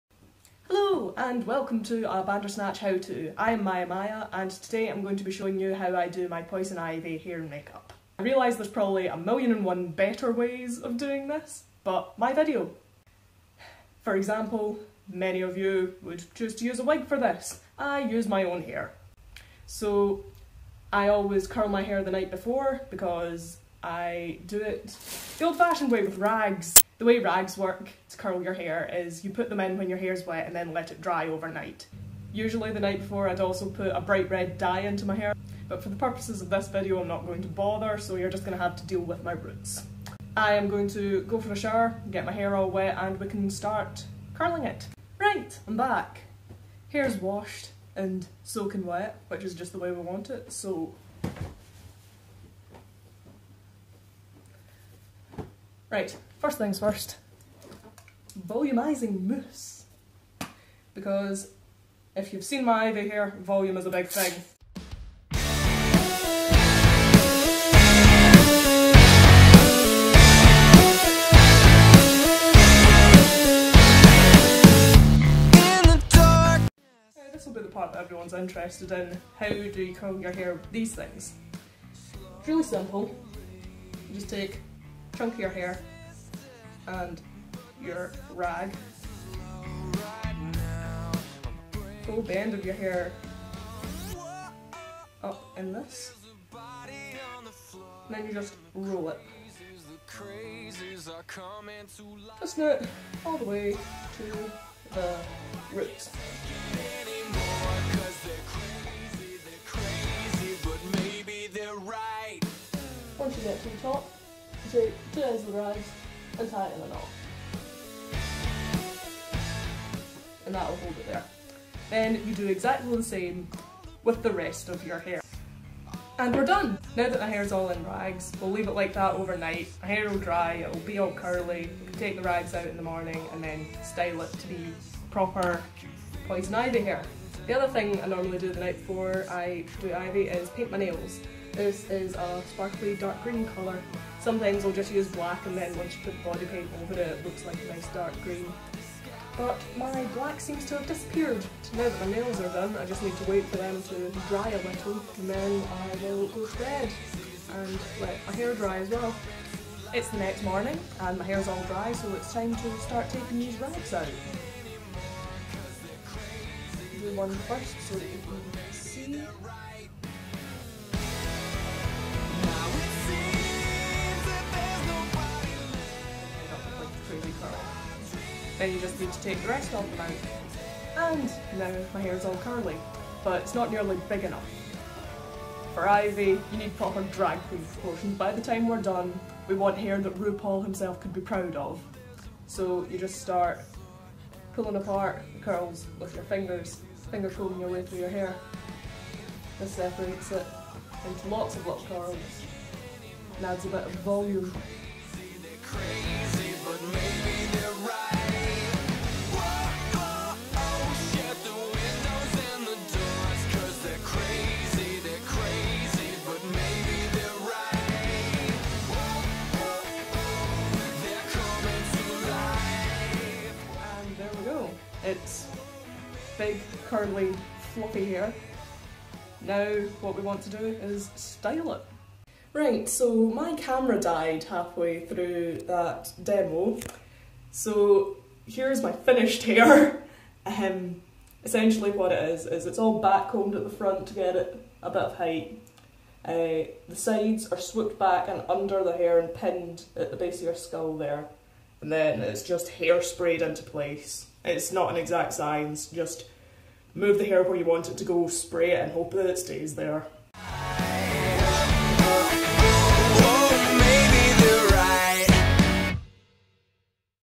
Becoming Ivy Part 1: Hair
In which Maya Maya demonstrates her Poison Ivy hairstyle.
Thumbnail: Photo by Fabulous Dandy, Editing by Psyclone Jack